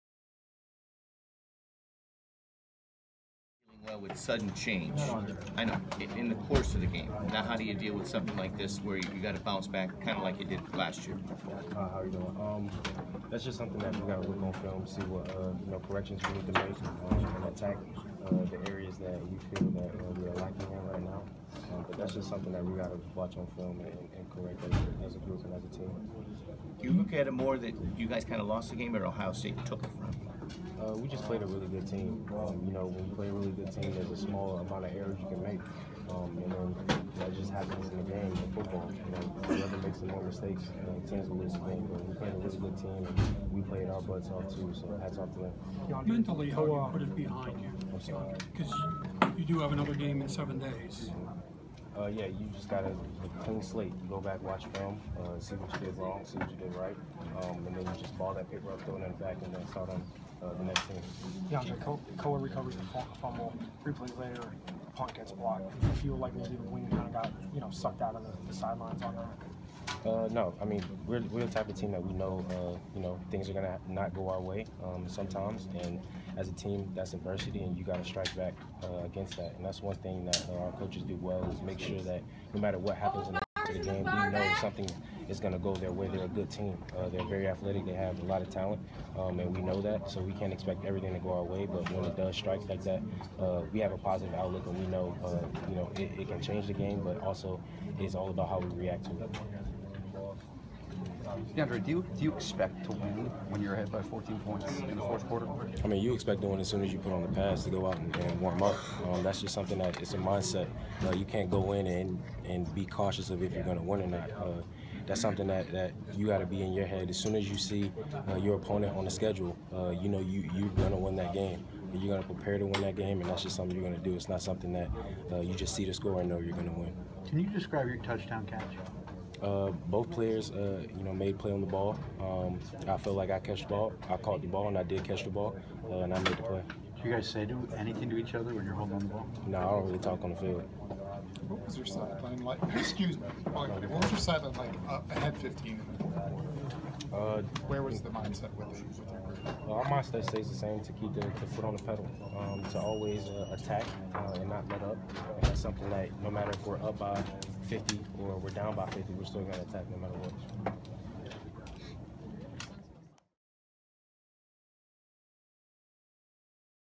DeAndre Thompkins: OSU Postgame
WR DeAndre Thompkins meets with the media following Penn State's 39-38 loss to Ohio State on Oct. 28, 2017.